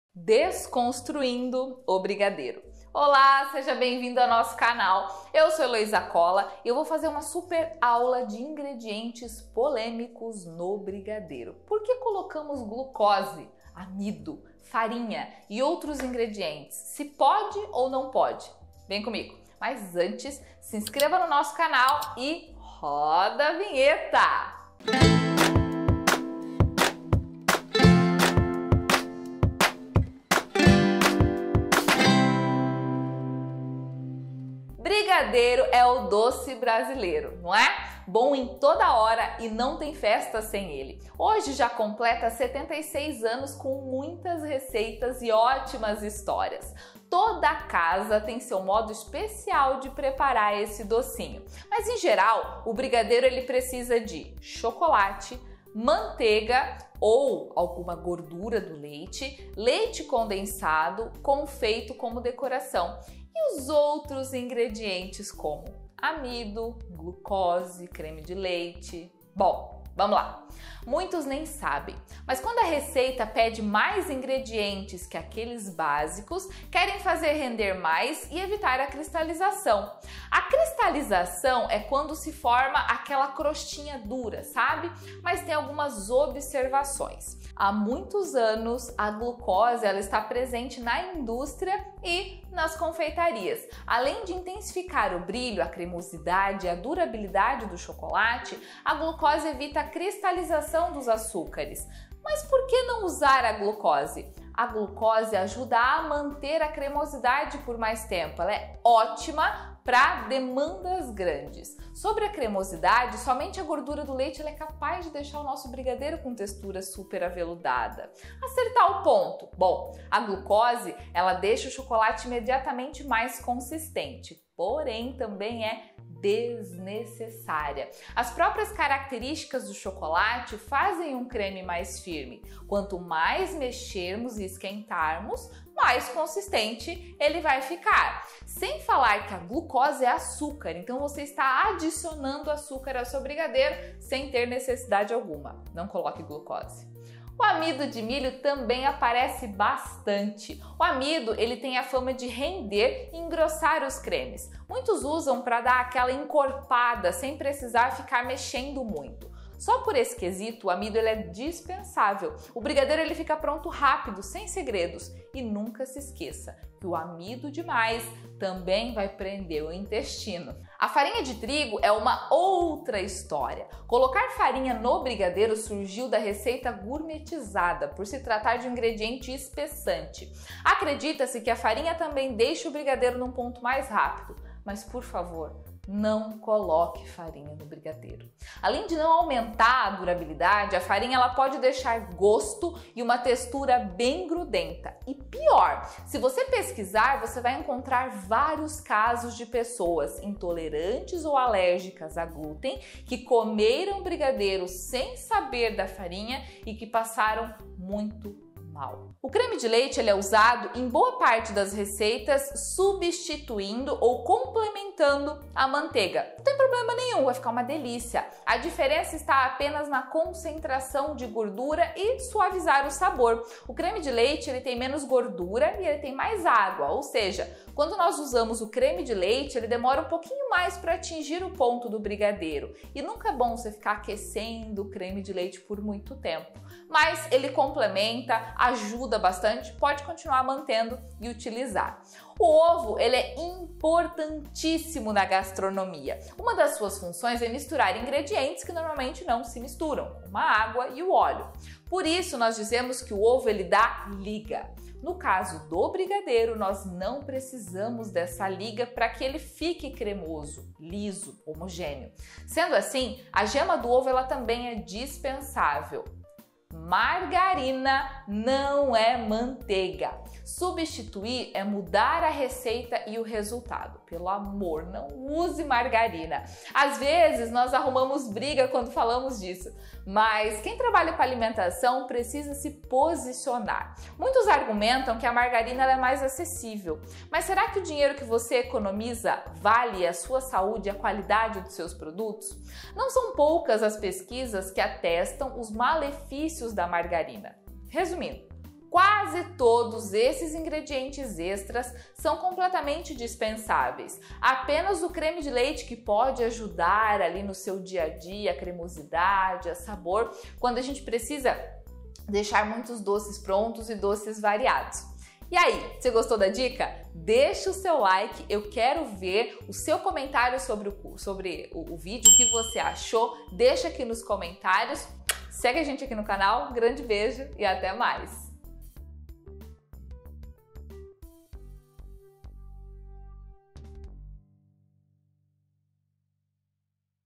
Polêmica do Brigadeiro | Render e não cristalizar | O que pode ou não colocar
O brigadeiro surgiu na década de 40, veja mais detalhes abaixo. Desde então, ganhou mais sabores, aromas e decorações, mas em geral o tradicional brigadeiro é o favorito.
Toda cozinheira gosta de dar o seu toque, não é? Algumas se baseiam só em preferência e outras em fundamentos técnicos de confeitaria. Foi daí que apareceram os primeiros brigadeiros gourmet que seriam doces mais bem trabalhados, digamos assim.
Adicionar glucose de milho, amido de milho, farinha de trigo, creme de leite ou margarina. Sempre pensando em fazer render mais e evitar a cristalização, quando forma aquela crostinha dura de brigadeiro velho, sabe?
Vamos ver o que pode ser colocado no brigadeiro e o que não pode!

Beijão da Elô

Direção, Edição e  Produção: Ana Carolina Cordeiro
Diagramação e Design: Rebeka Devai
Roteiro: Celinne Nishimura